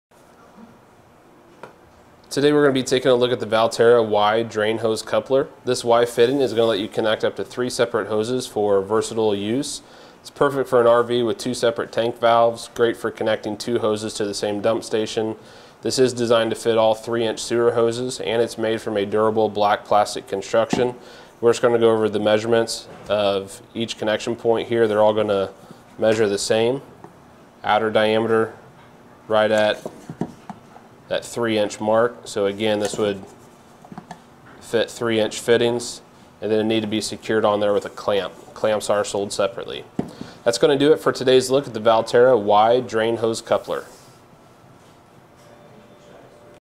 etrailer | In-Depth Review of the Valterra "Y" Coupling for RV Sewer Hose
Click for more info and reviews of this Valterra RV Sewer Hose Fittings:

Today, we're going to be taking a look at the Valterra "Y" Drain Hose Coupler. This Y fitting is going to let you connect up to 3 separate hoses for versatile use. It's perfect for an RV with 2 separate tank valves, great for connecting 2 hoses to the same dump station. This is designed to fit all 3-inch sewer hoses, and it's made from a durable, black, plastic construction. We're just going to go over the measurements of each connection point here, they're all going to measure the same. Outer diameter, right at that 3-inch mark, so again, this would fit 3-inch fittings, and then it would need to be secured on there, with a clamp.

Clamps are sold separately. That's going to do it for today's look at the Valterra "Y" Drain Hose Coupler.